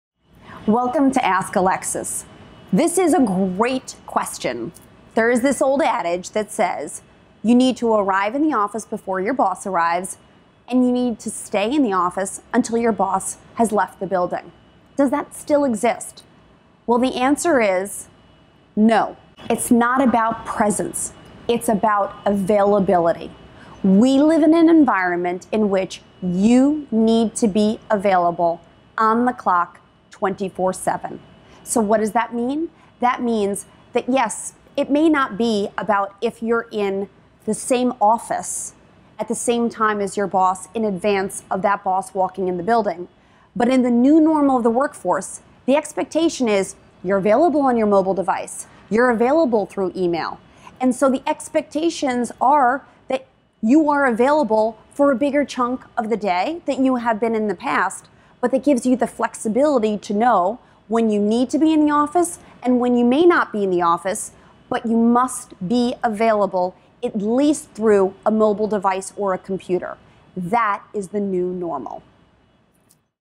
Becoming More Available
Becoming more available gives you the flexibility to know when you need to be in the office and when you may not be in the office. You must always be available through a mobile device or a computer.